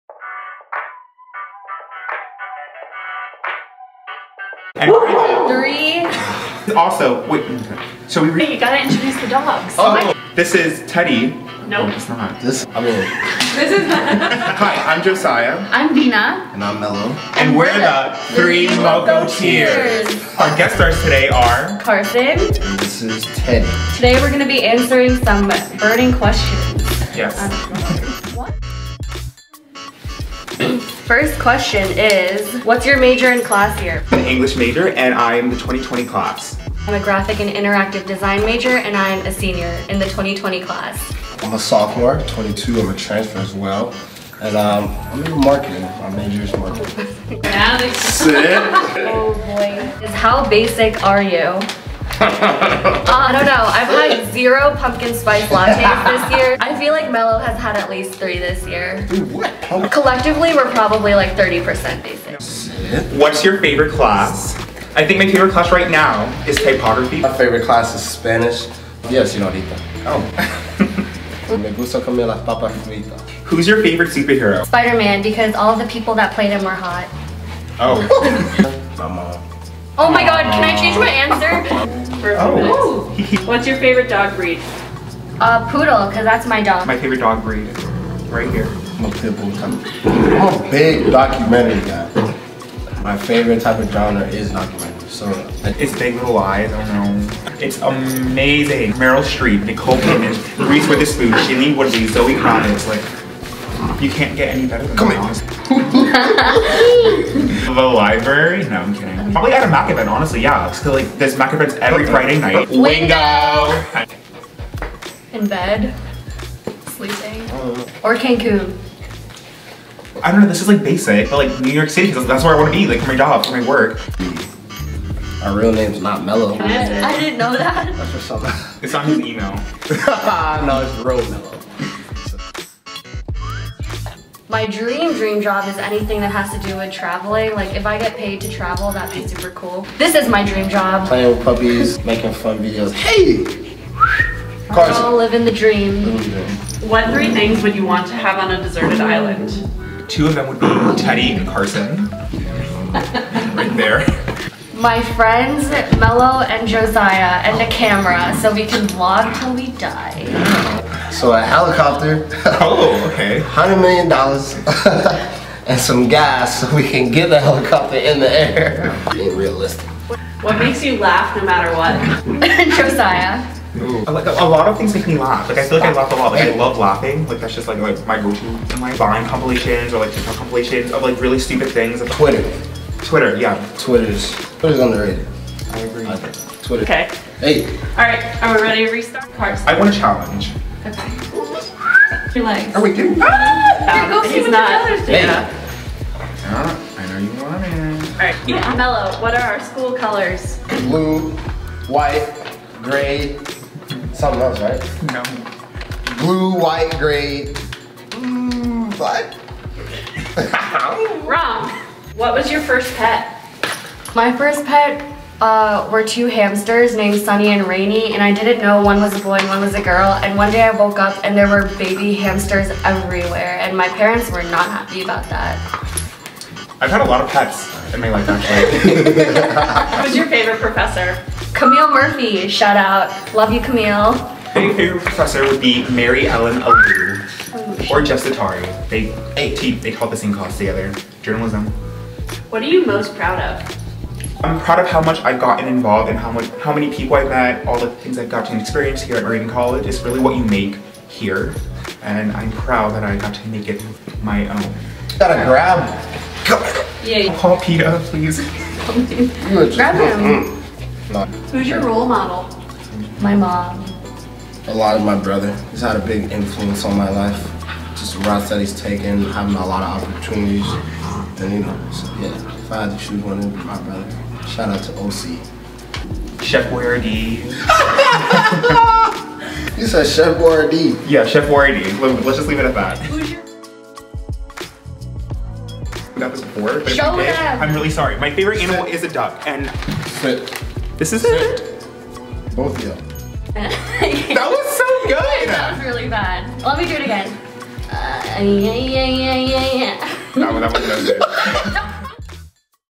MEET OUR STUDENT VLOGGERS: THE THREE MOCOTEERS | Moravian College
Our official student vloggers, Josiah, Vina and Mello, answer some fun and burning questions while playing with the cutest campus pups around, Carson & Teddy!

Shout-out to Keith and John for letting their hounds be featured in this video!